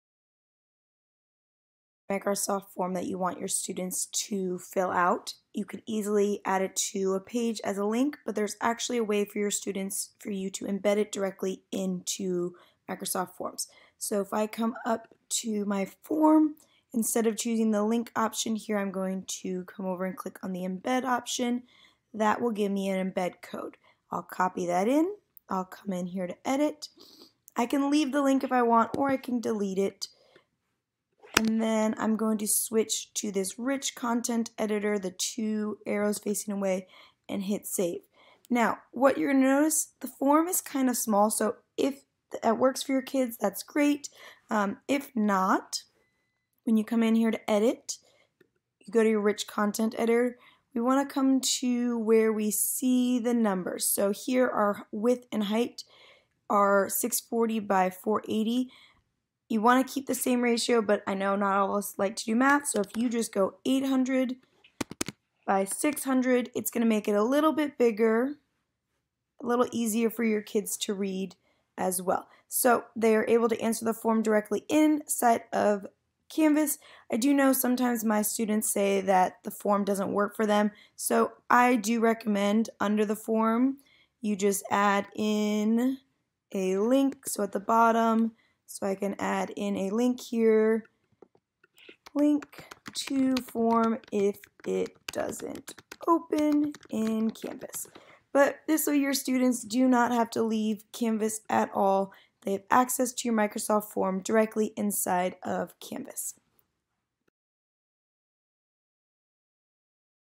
Microsoft Form - Embedding in Canvas
This video shows you how to embed a Micrososft Form into a Canvas Page and how to change the size of the form within Canvas.